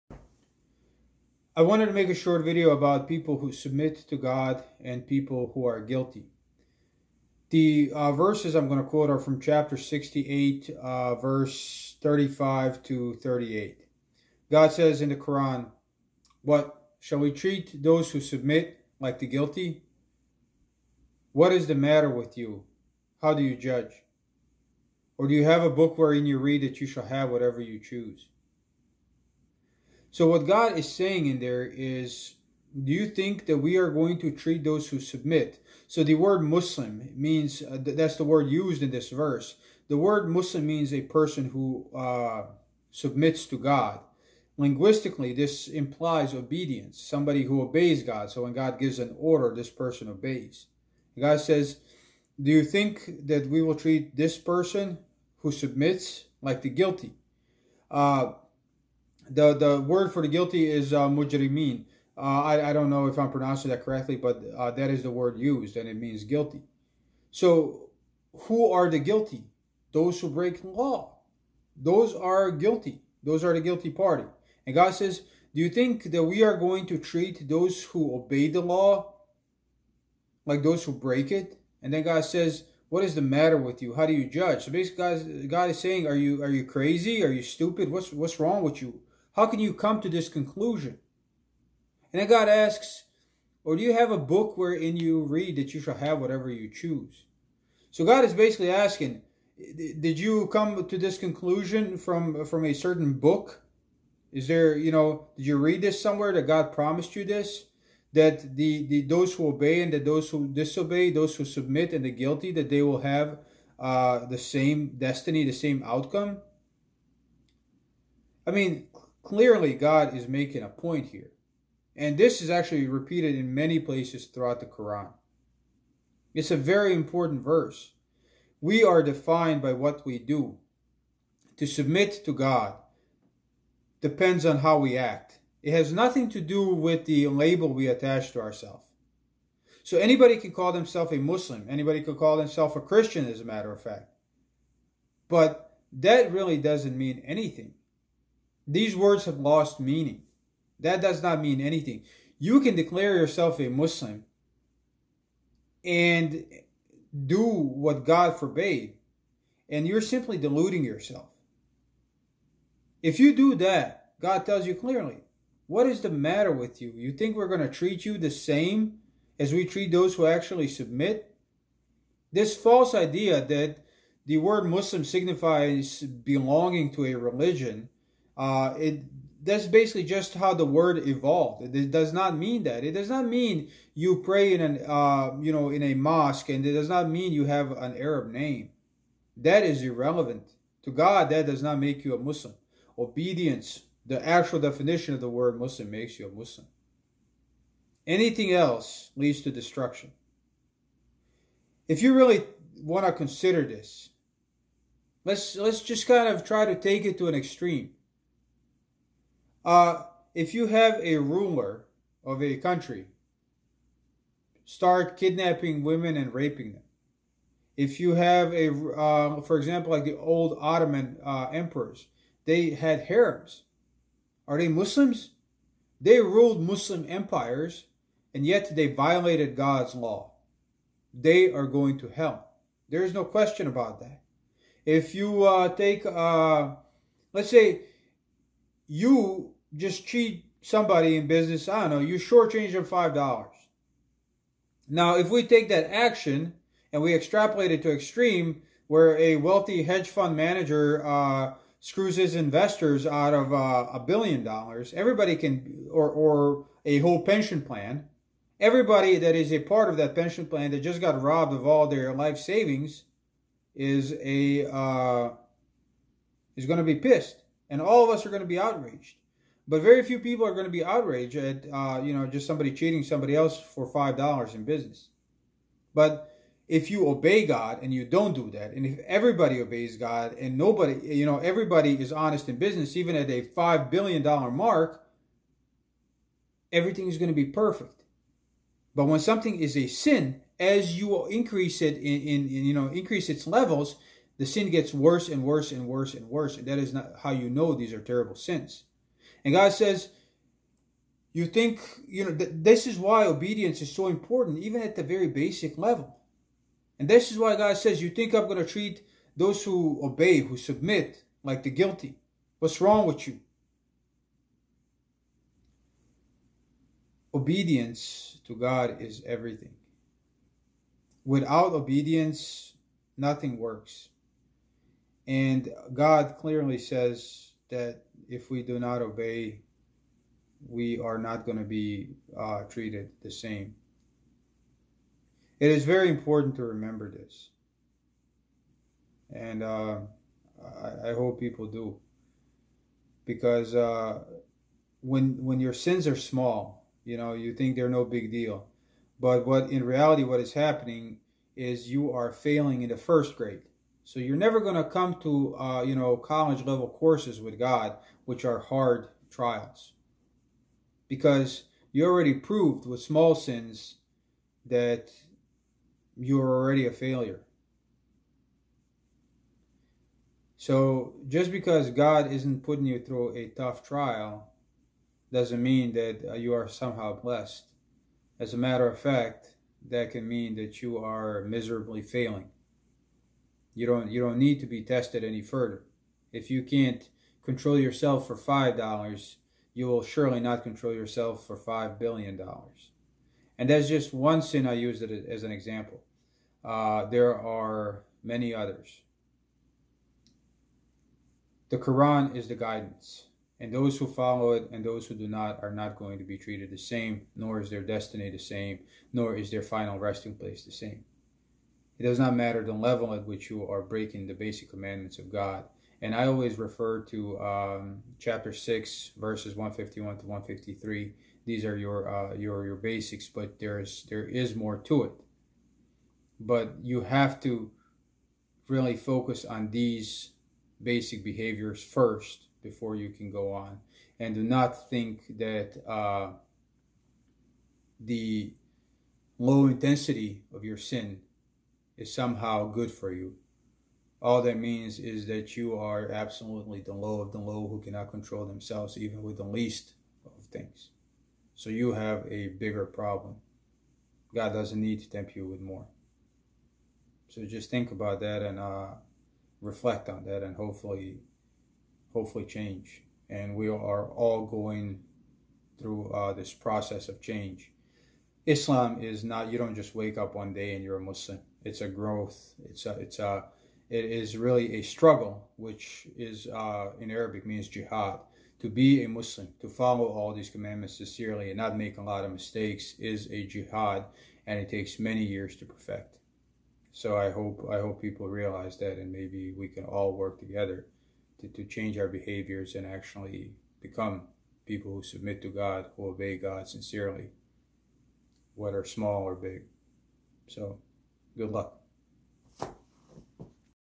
Shall We treat those who submit as the guilty?
"Shall We treat those who submit as the guilty? What is the matter with you, how do you judge? Or do you have a book wherein you read that you shall have whatever you choose?" - Quran 68:35-38